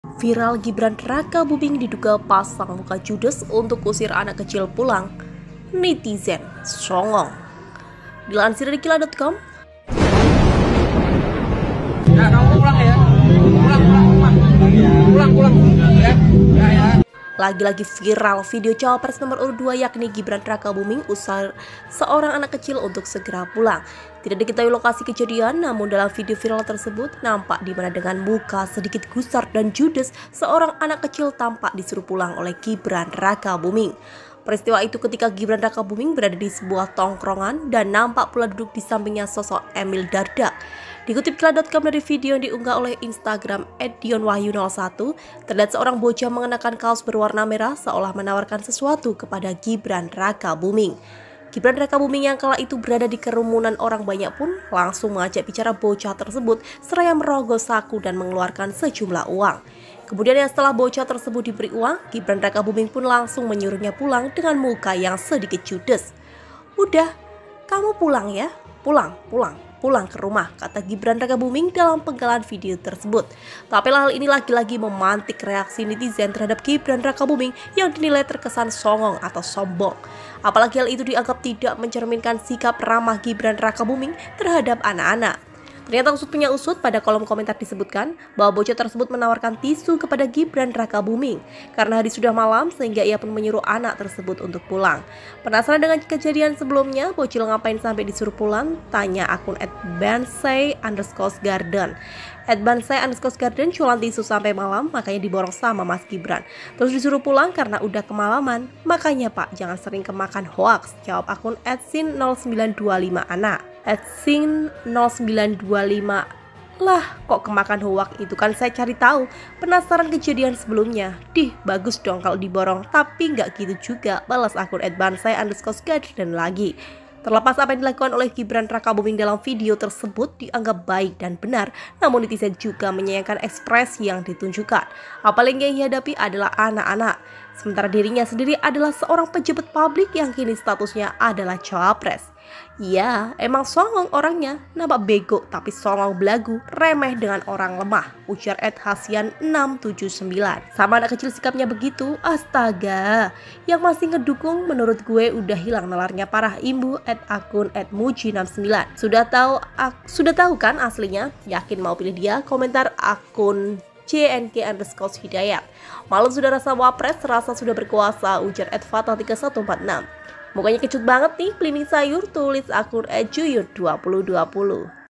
Viral Gibran Rakabumimg Diduga Pasang Muka Judes untuk Usir Anak Kecil Pulang, Netizen: Songong!
Lagi-lagi viral video cawapres nomor urut 2 yakni Gibran Rakabuming usir seorang anak kecil untuk segera pulang.

Tidak diketahui lokasi kejadian, namun dalam video viral tersebut, nampak dimana dengan muka sedikit gusar dan judes seoarang anak kecil tampak disuruh pulang oleh Gibran Rakabuming.

Peristiwa itu ketika Gibran Rakabuming berada disebuah tongkrongan, dan nampak pula duduk disampingnya sosok Emil Dardak.